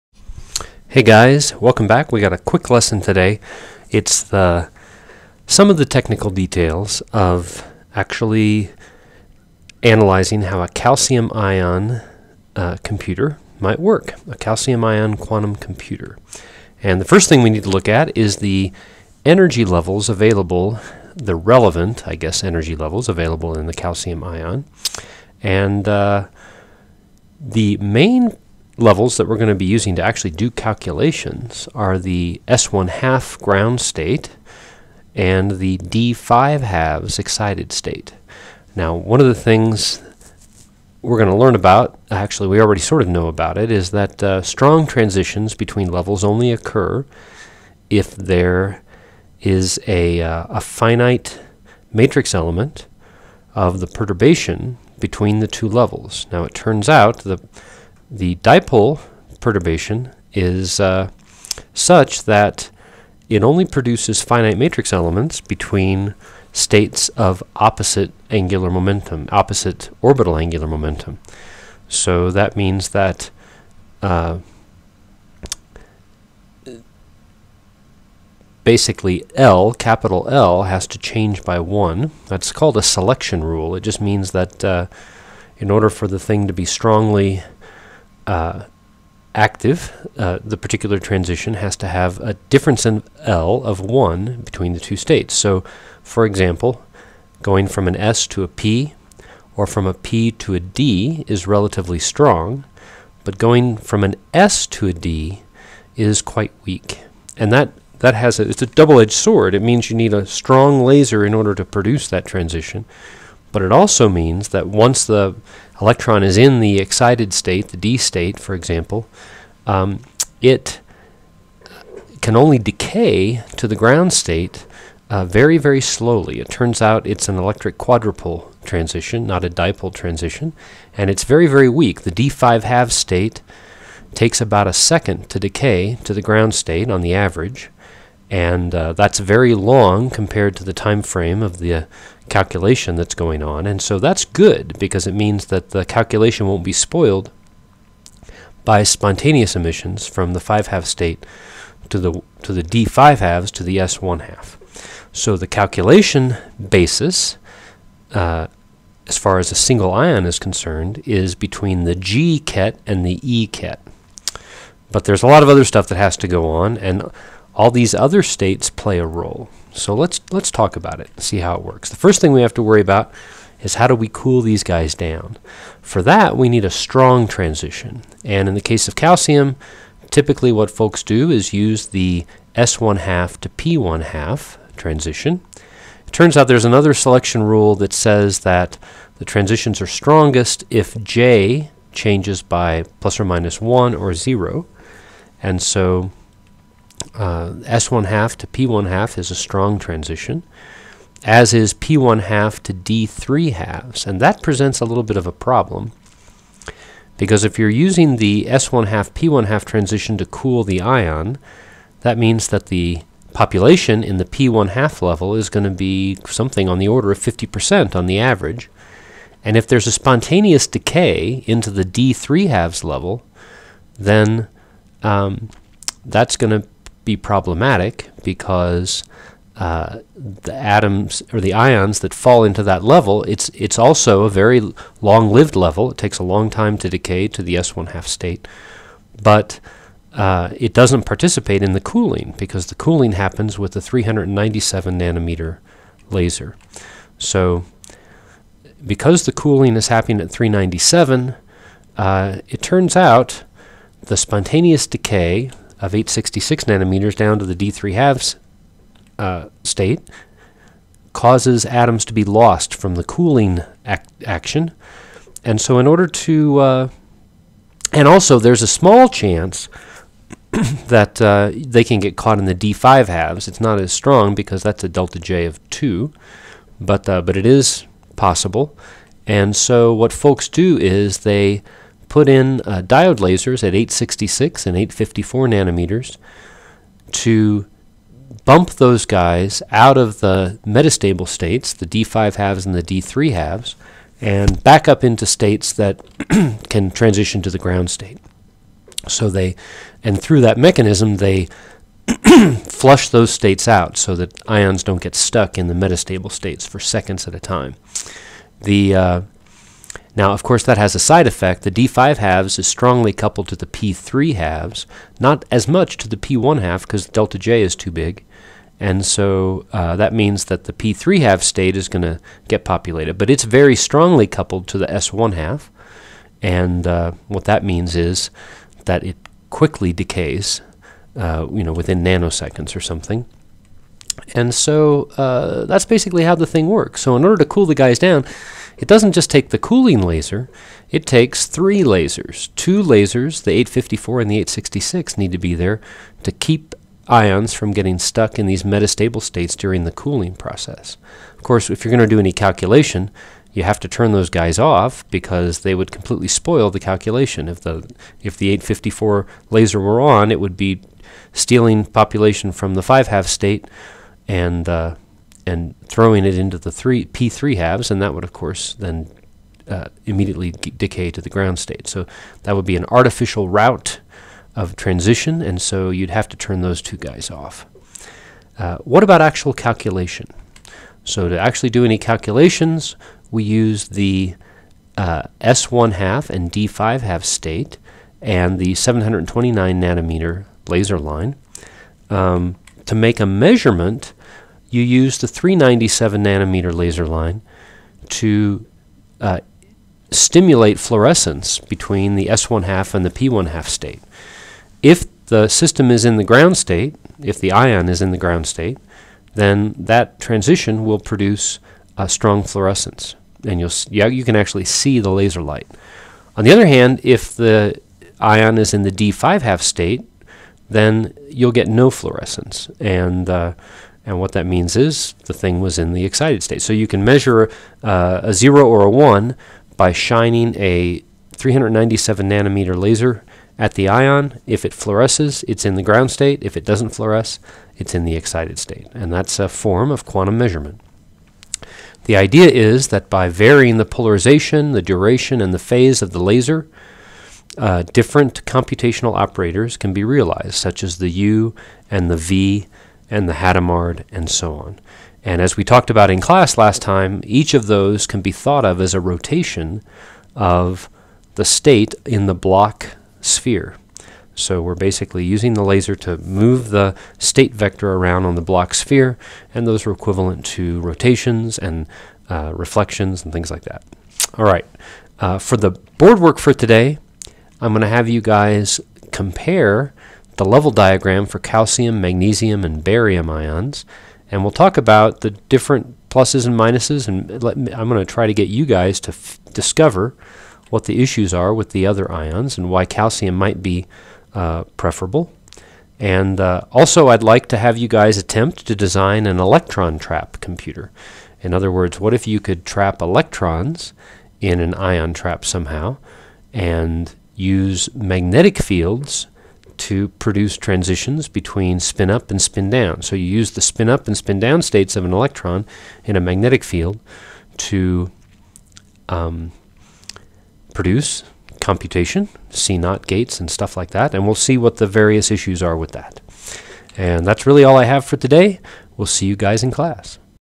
Lesson25: Ca+ Ion Trap Computer
Basic description of the details of running an ion trap computer using Ca+ ions. Energy level scheme of Ca+ is described as it relates to cooling and computing with Ca+ ions in a linear Paul trap. Pre class slides by Steve Spicklemire.